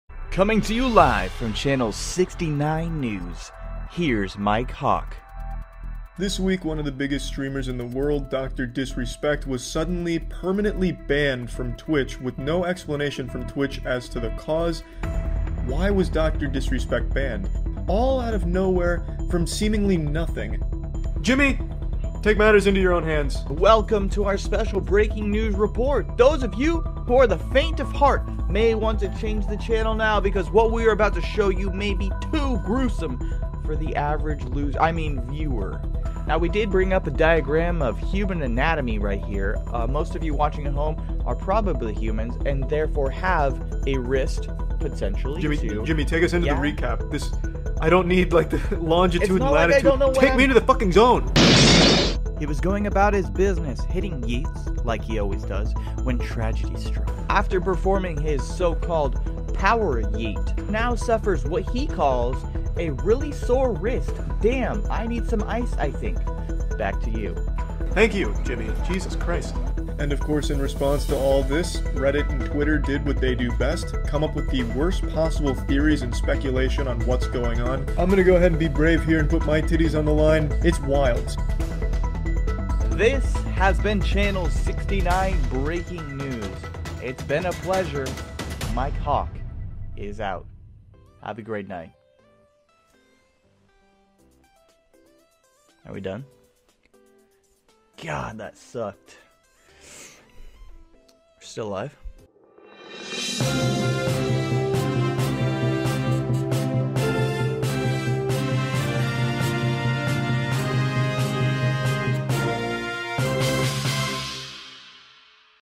TWITCH STREAMER BANNED- CHANNEL SIXTY NINE BREAKING NEWS
Channel sixty-nine news putting our ta-tas on the line.

this is fake news and any of the topics discussed should be taken as parody.

please yeet responsibly.